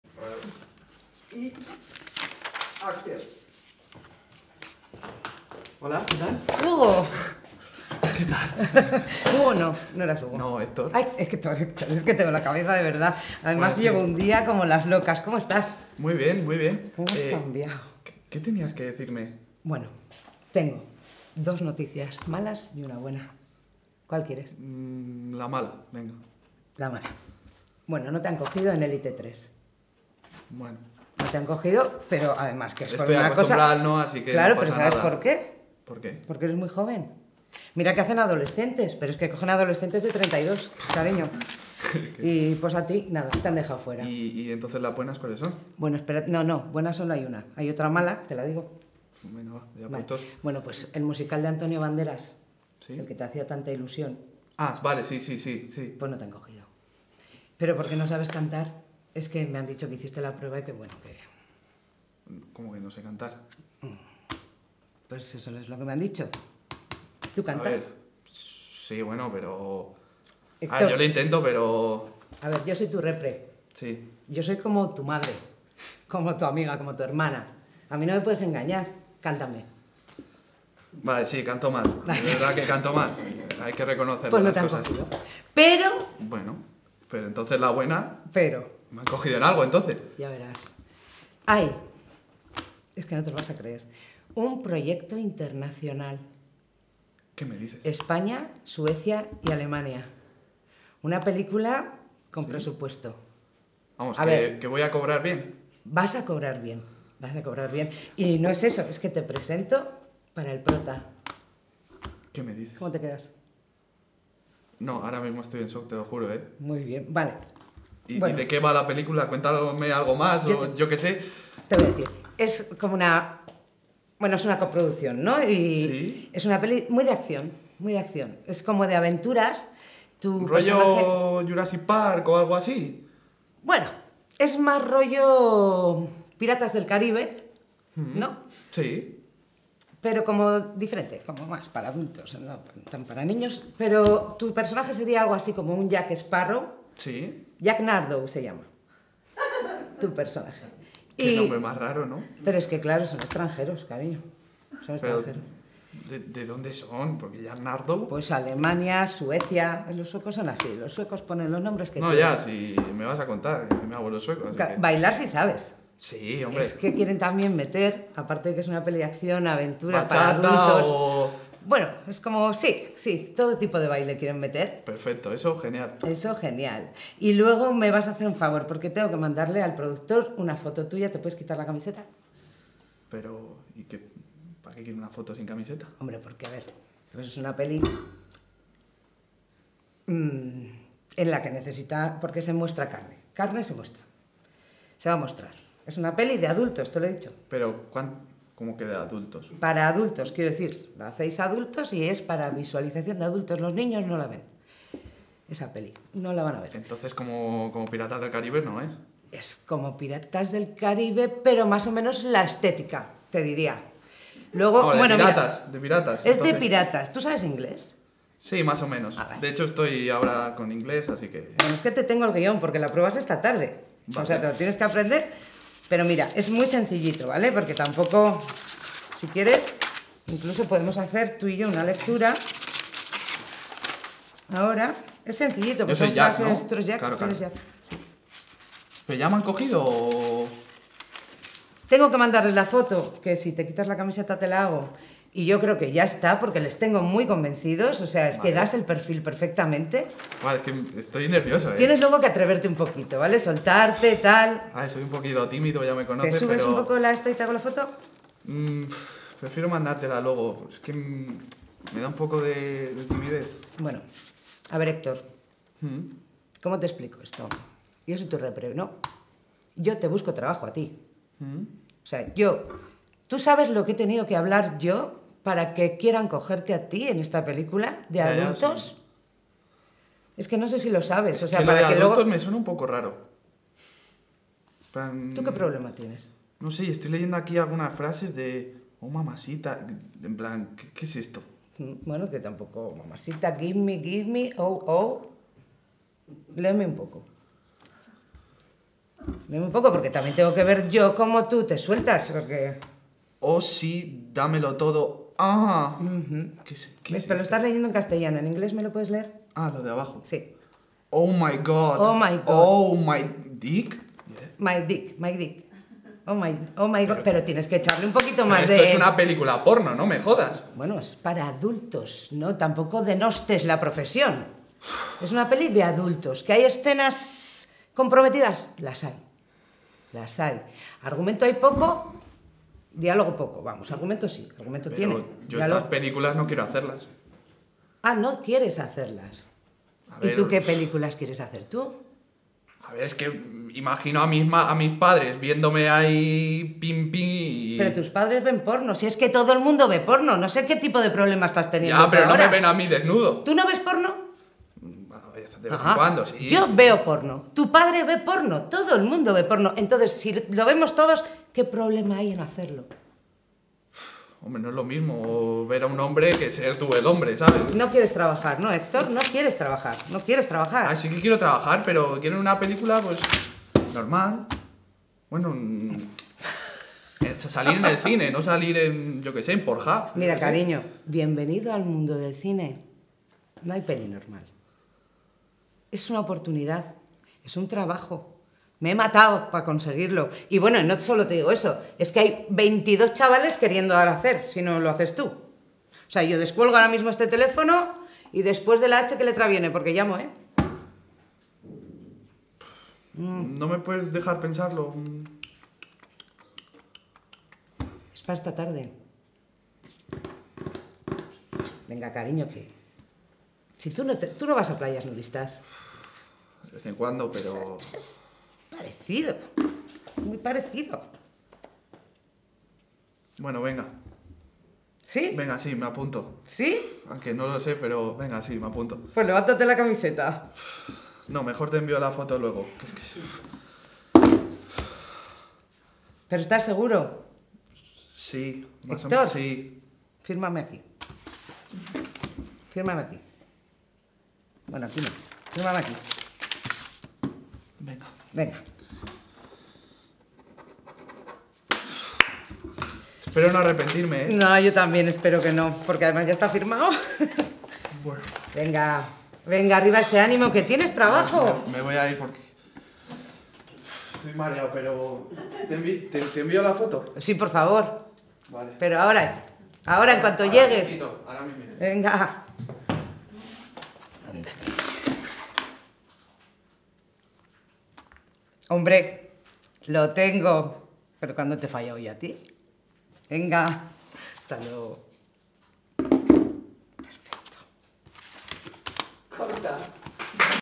cine para adultos.impro.
taller de Gracia querejeta en escuela de actores.